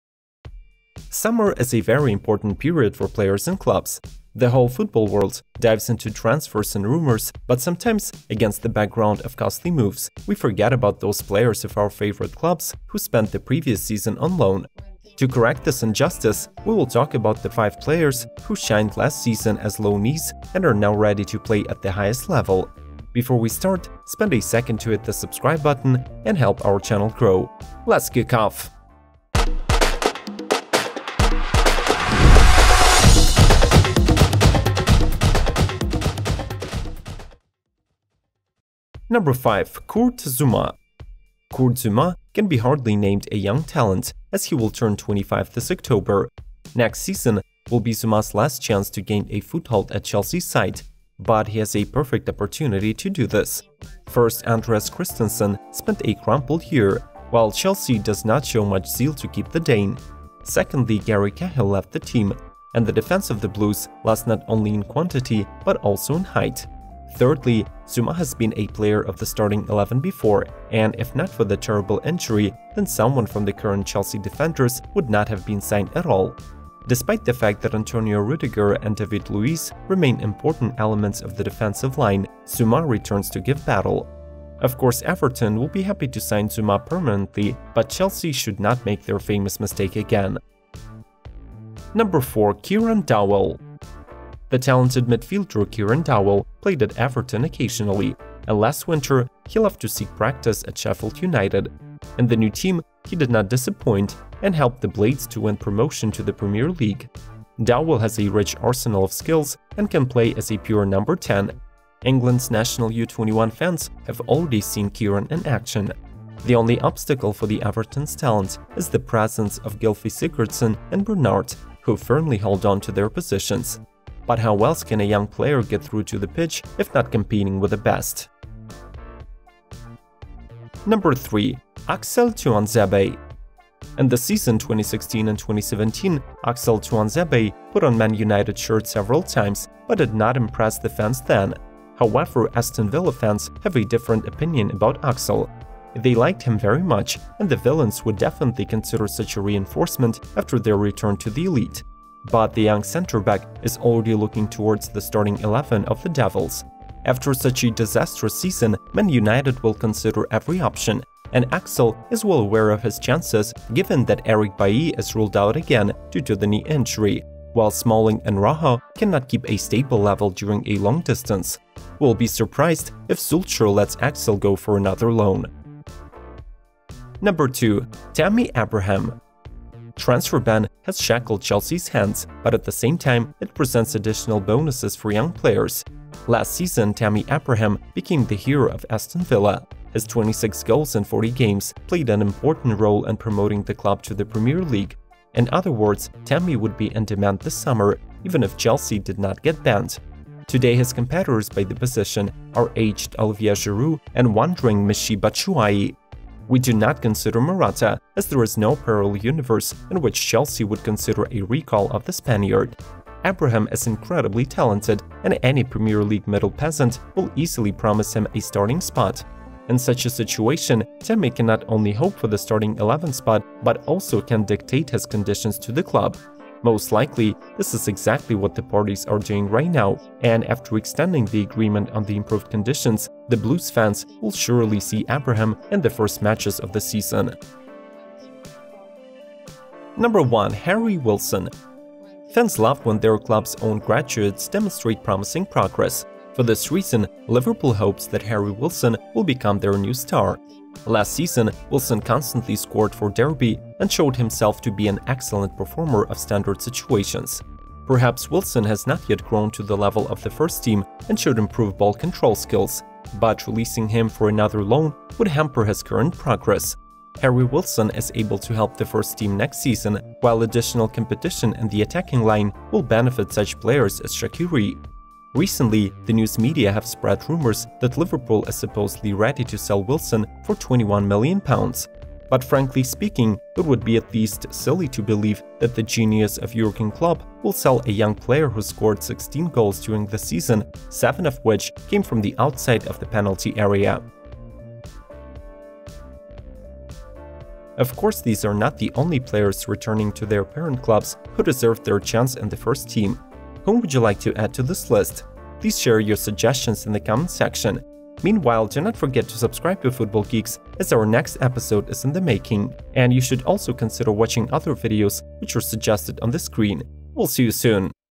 Top 5 Loan Players Who Will Break Into the First Team to Star in the Premier League 2019/2020
Summer is a very important period for players and clubs. The whole football world dives into transfers and rumors, but sometimes against the background of costly moves, we forget about those players of our favorite clubs who spent the previous season on loan. To correct this injustice, we will talk about 5 players who have shined last season as loanees and are now ready to play at the highest level.

FG_PremierLeague